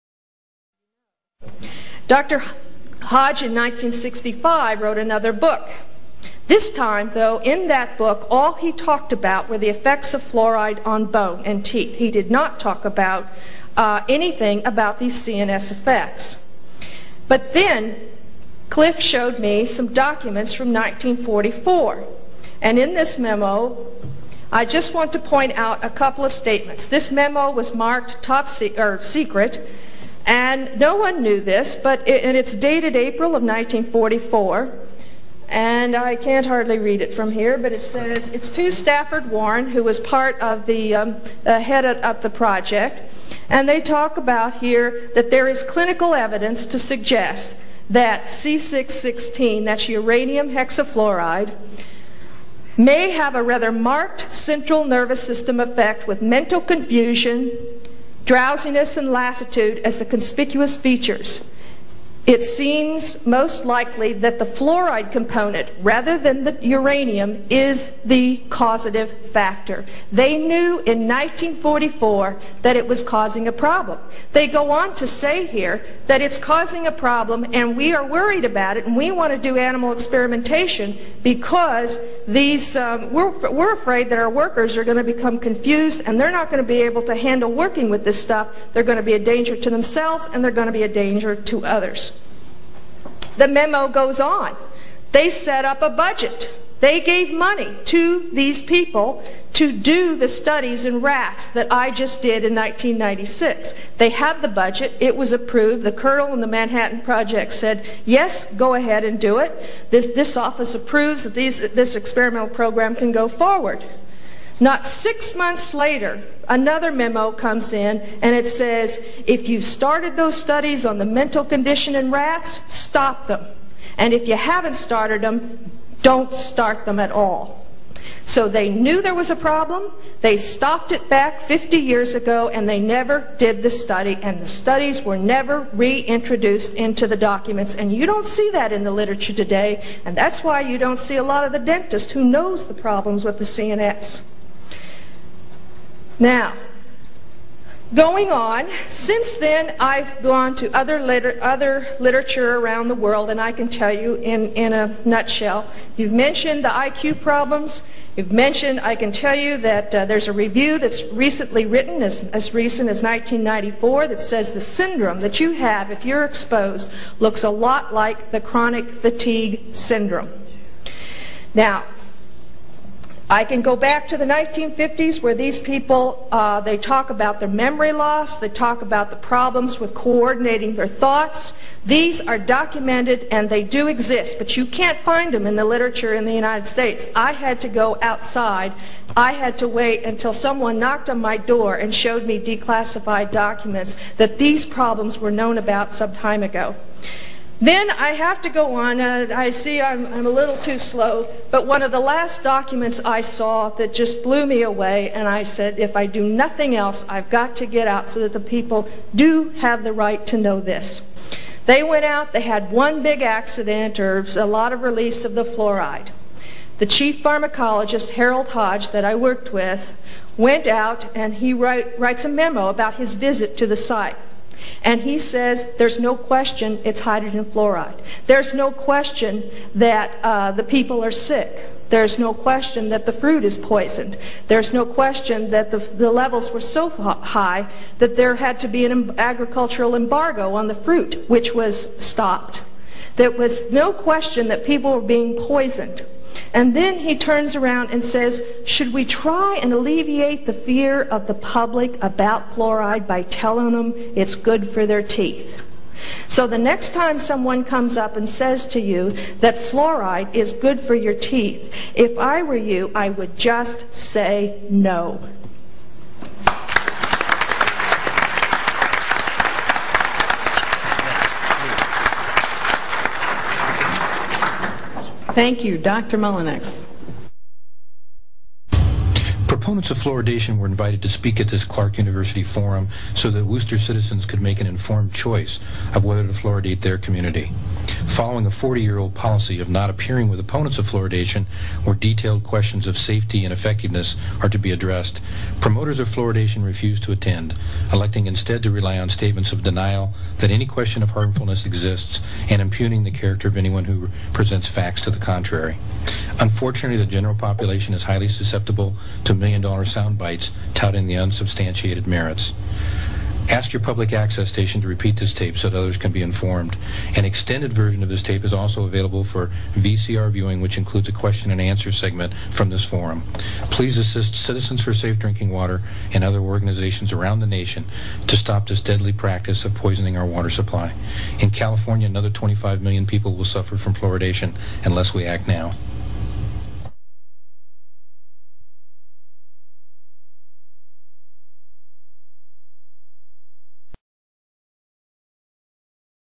Fluoride Expert Dr. Phyllis Mullinex on the Dangers of Water Fluoridation (3/3)
Dr. Phyllis Mullinex was head of the department of toxicology at Forsyth Dental Research Institute for eleven years and is one of the leading experts in the field of fluoride research and it's harmful effects on human physiology.

"And if care was not taken in the breeding, your dogs and birds would greatly deteriorate? Certainly. And the same of horses and animals in general? Undoubtedly. Good heavens! My dear friend, I said, WHAT CONSUMATE SKILL WILL OUR RULERS NEED IF THE SAME PRINCIPLE HOLDS OF THE HUMAN SPECIES? Certainly the same principle holds, but why does this involve any particular skill? Because, I said, OUR RULERS WILL OFTEN HAVE TO PRACTICE UPON THE BODY CORPORATE [POPULATION] WITH MEDICINES. Now you know that when patients do not require medicines, but have only to be put under a regimen, the inferior sort of practitioner is deemed to be good enough; but when medicine has to be given the doctor should be more of a man. I mean, I replied, that OUR RULERS WILL FIND A CONSIDERABLE DOSE OF FALSEHOOD AND DECEIT NECESSARY FOR THE GOOD OF THEIR SUBJECTS. We were saying that the use of all these things regarded as medicines might be of advantage. And we were very right. And this lawful use of them seems likely to be often need in the regulation of BIRTHS  and MARRIAGES. [a.k.a. EUGENICS]" ~Plato's Republic, Book 5, pages 161-162